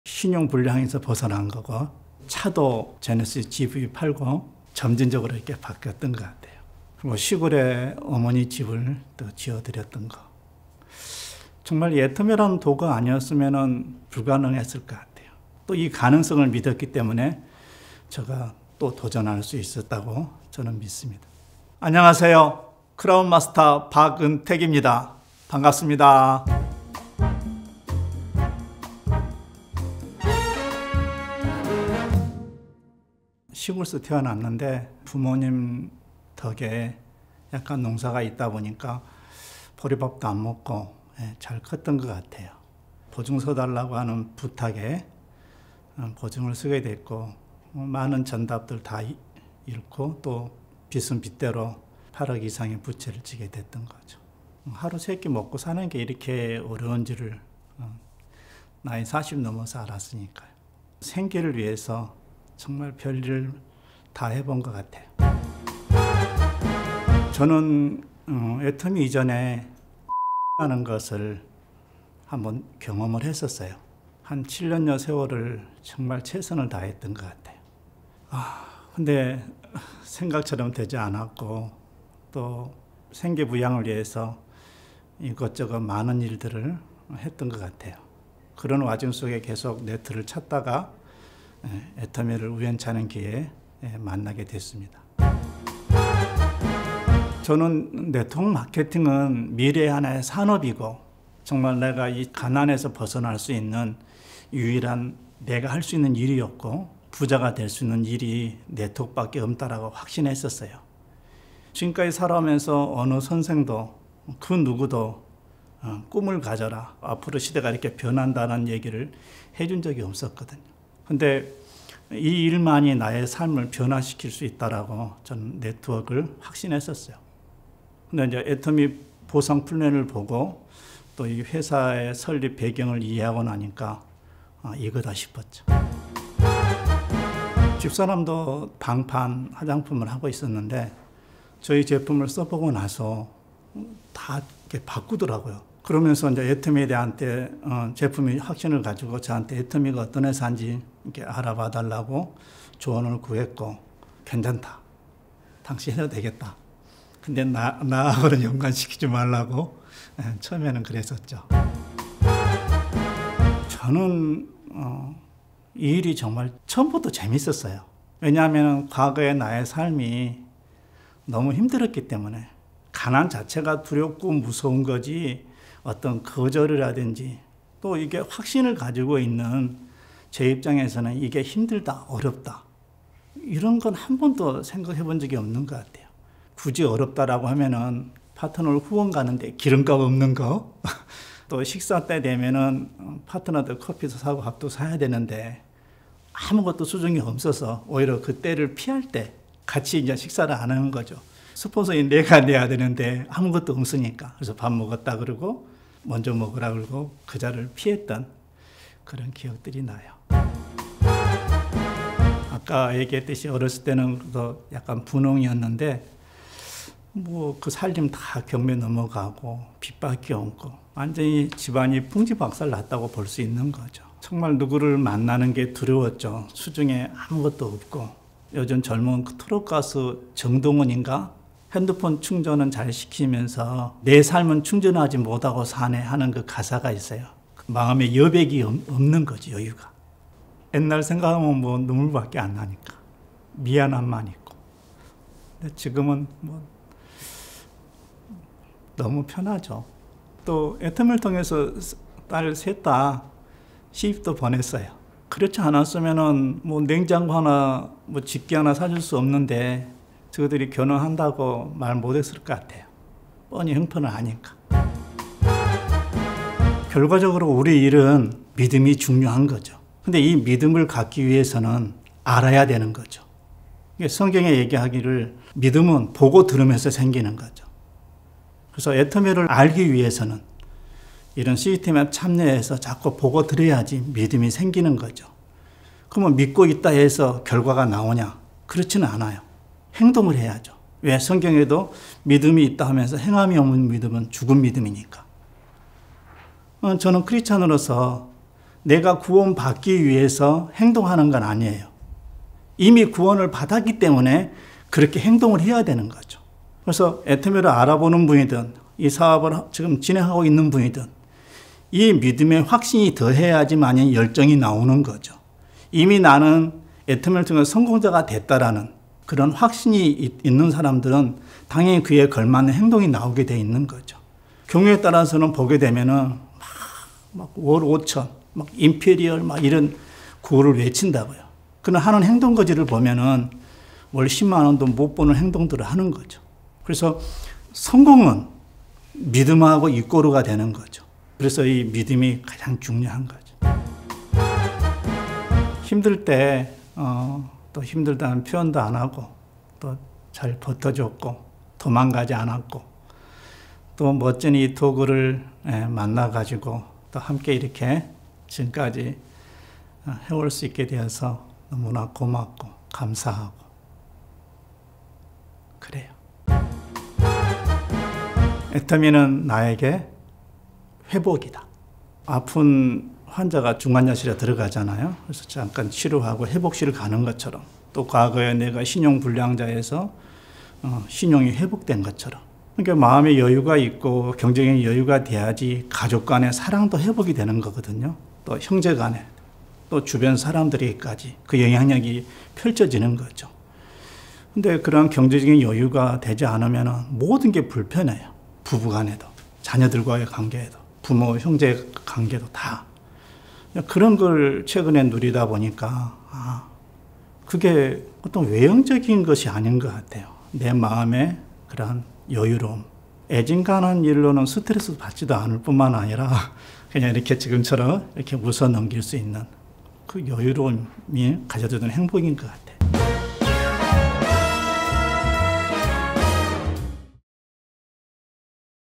[TheATOMIANS] The ATOMIANS Ep.36ㅣ못 걷는자가 걸으며ㅣ@박은택CM
00:00 intro
00:34 애터미 이전의 삶은 어떠셨나요?
01:06 애터미를 알게 된 계기는?
01:36 애터미 사업을 결심하게 된 계기는?
02:17 아내분의 반응은 어땠나요?
02:42 사업을 하면서 힘든 적은 없었나요?
03:31 애터미를 만나고 나서 삶의 변화가 있다면?
04:26 초기 사업자분들을 위한 팁이 있다면?
05:44 아내 이경자CM에게 보내는 영상편지
06:09 ‘애터미는 나에게 ( )이다’

디 애터미언즈 Ep.35

전세계 모든 애터미언들의
잘살고, 사랑하며, 배우고 공헌하는
균형잡힌 삶의 이야기를 들어보는 시간!
TheATOMIANS 그 서른 여섯 번째 이야기

‘확신이 있는 사람은 그에 걸맞은 행동이 나오게 되죠’

고객만족을 넘어 고객성공을 실현하는 애터미 코리아의  공식 유튜브 채널입니다.